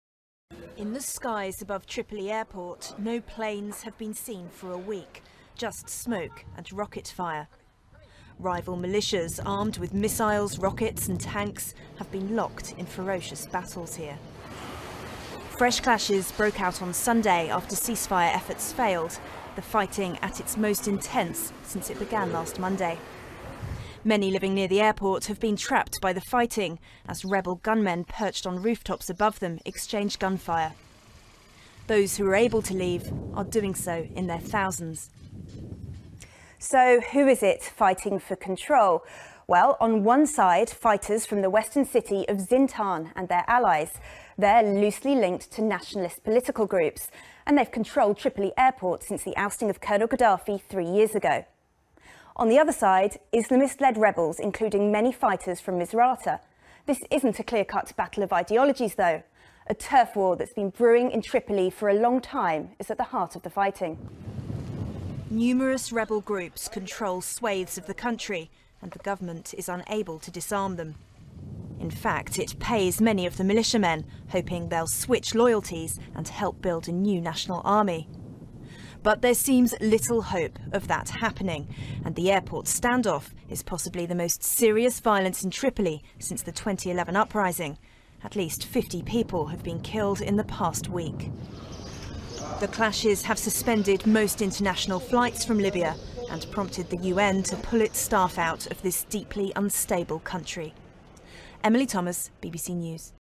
Libya militias in renewed battle for airport
At least 50 people have been killed in fighting at Tripoli airport since it began a week ago, according to the health ministry.

More heavy fighting between rival militias broke out in and around the main international airport on Sunday.

The violence has forced the airport to close and the Libyan authorities have already warned that repairs will take months.

Emily Thomas reports.